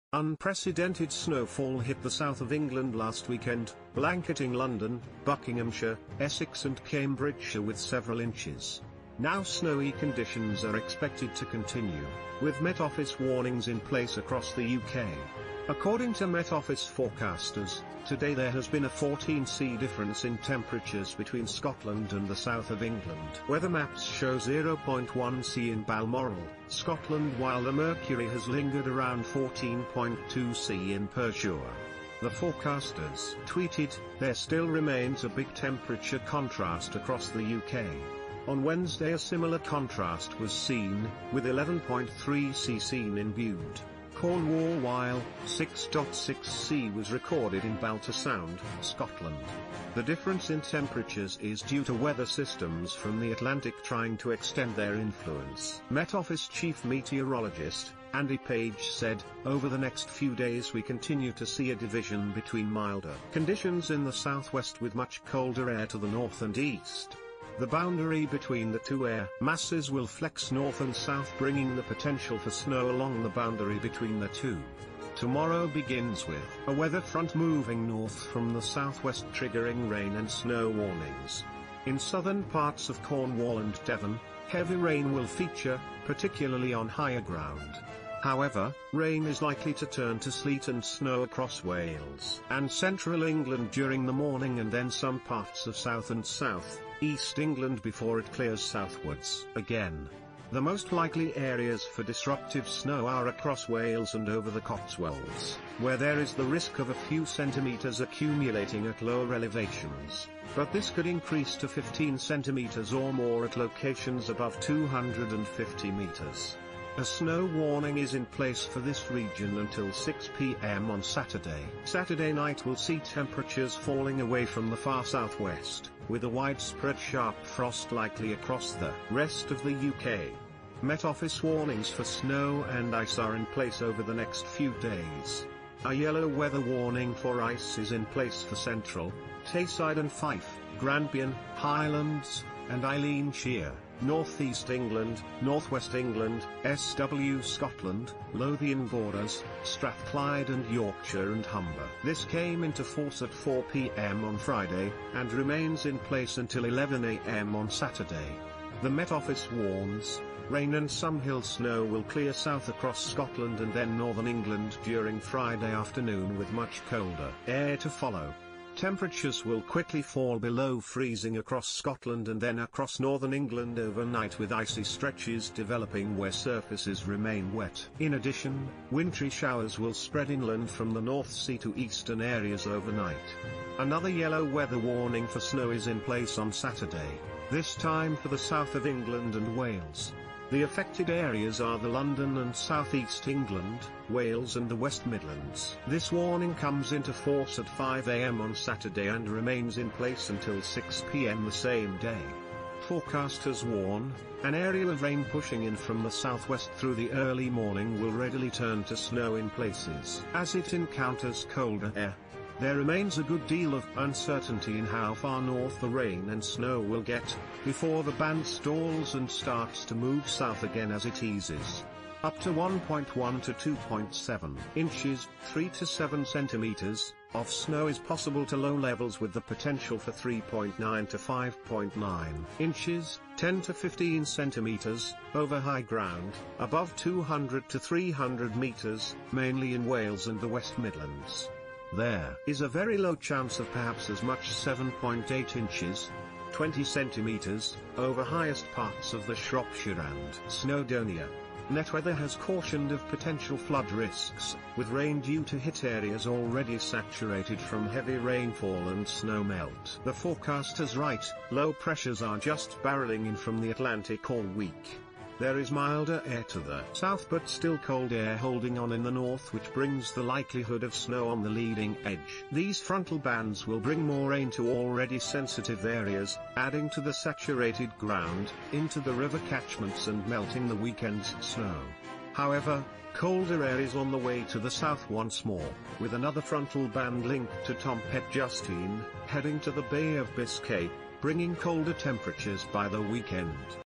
Met Office snow warnings: Scandinavian blast to bring 8 inches snow- mercury sinks to -10C
Unprecedented snowfall hit the south of England last weekend, blanketing London, Buckinghamshire, Essex and Cambridgeshire with several inches. Now snowy conditions are expected to continue, with Met Office warnings in place across the UK.  According to Met Office forecasters, today there has been a